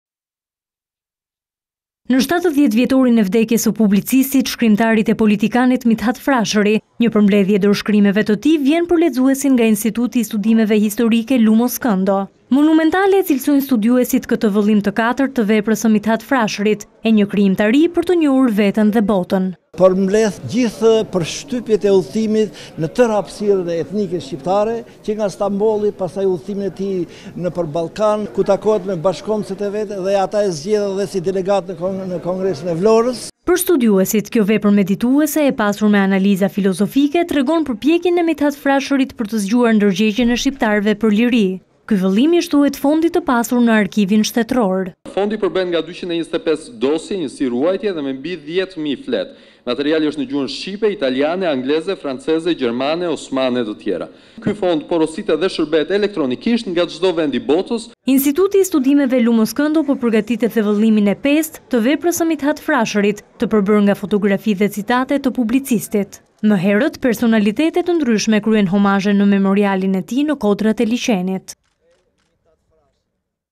Publikohen doreshkrimet e Mit'hat Frasherit
Ne 70 vjetorin e vdekjes së publicistit, shkrimtarit e politikanit Mit’hat Frashëri, një përmbledhje e dorëshkrimeve të tij vjen për lexuesin nga Instituti i Studimeve Historike “Lumo Skëndo”. Monumentale e cilësojnë studiuesit këtë vëllim të 4 të veprës së Mithat Frashërit, e një krijimtari për të njohur veten dhe botën.

Për studiuesit kjo vepër medituese, e pasur me analiza filozofike, tregon përpjekjen e Mithat Frashërit për të zgjuar ndërgjegjen së shqiptarëve për liri. Ky vëllim i shtohet fondit të pasur në Arkivin shtetëror.

Instituti i Studimeve “Lumo Skëndo” po përgatit edhe vëllimin e 5-të të veprës së Mit’hat Frashërit, të përbërë nga fotografi dhe citate të publicistit. Më herët, personalitete kryen homazhe në memorialin e tij në Kodrat e Liqenit.

Tv Klan